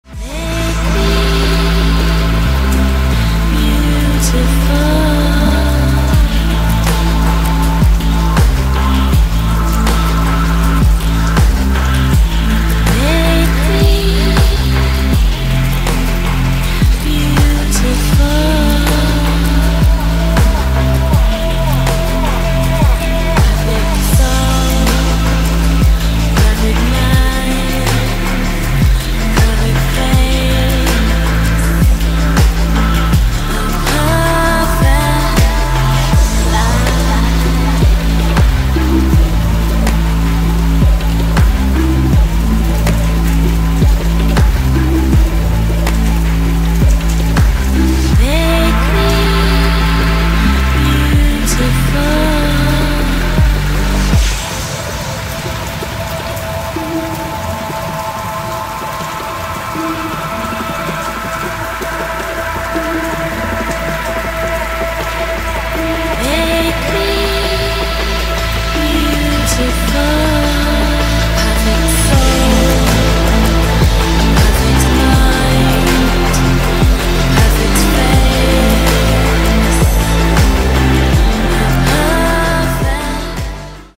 dance in the rain | FUJIFILM X-T3 + Sigma 18-35 | Cinematic Video
Танцевать под дождем еще то испытание, ведь тебе затекает вода куда угодно, особенно в уши и ты не слышишь музыку, а соответсвенно приходится танцевать по памяти.